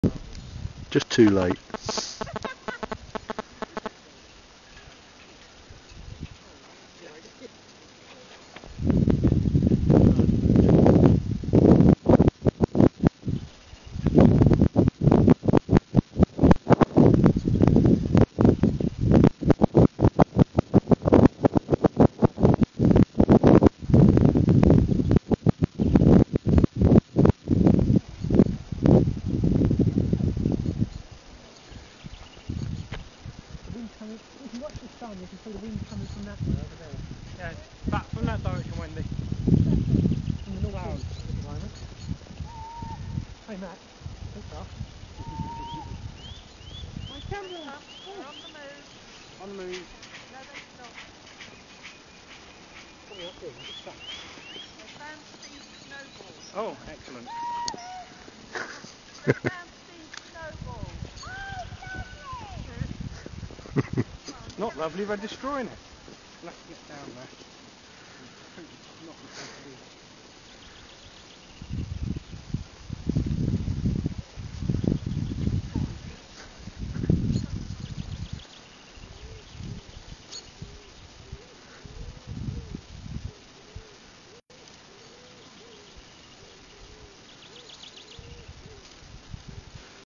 Young Wolves first Snow
Torak, Mosi & Mai's first view of snow at just under 2 years old